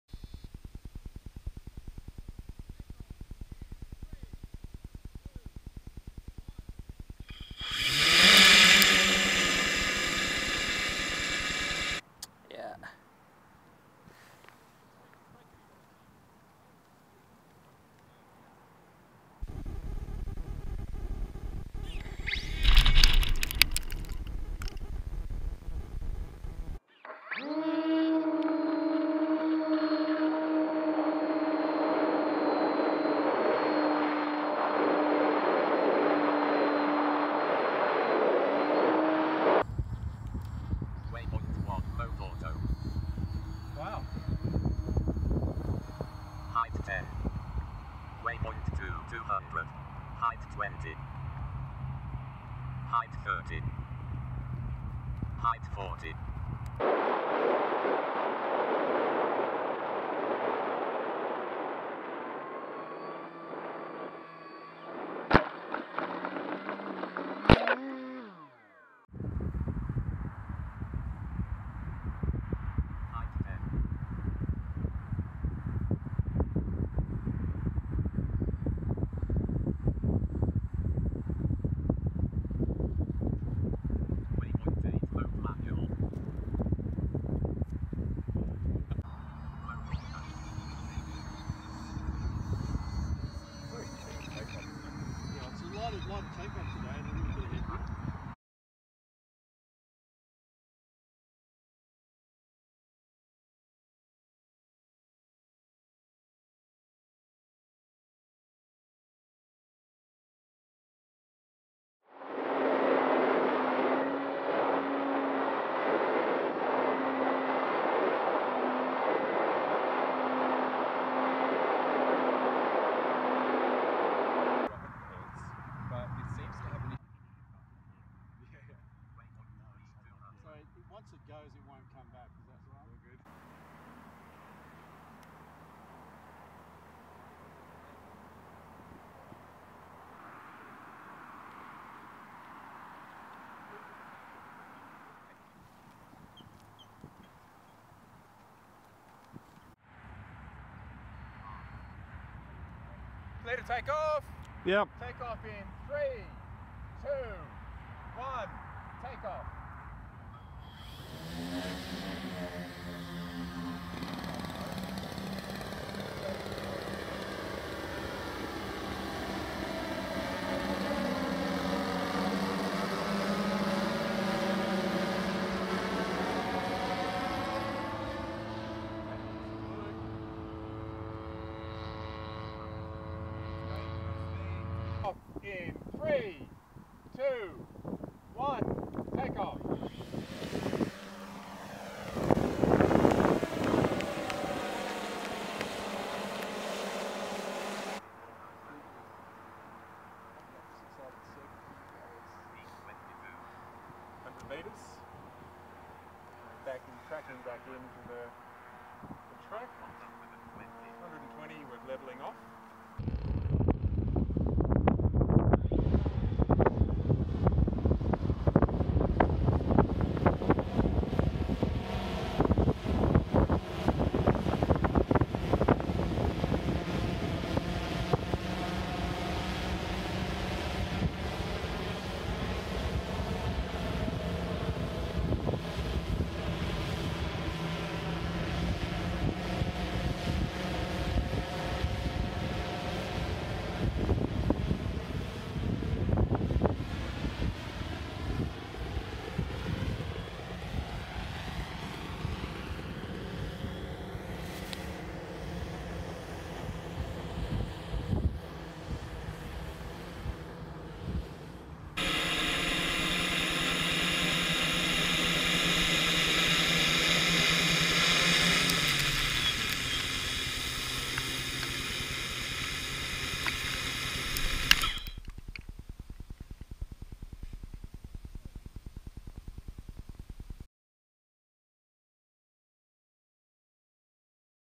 CanberraUAV - 2018 UAV Challenge D3
CanberraUAV - 2018 UAV Challenge D3. The FliteTest Kraken Comms Relay aircraft and the OctoPorter search and recovery aircraft in action with the ground stations.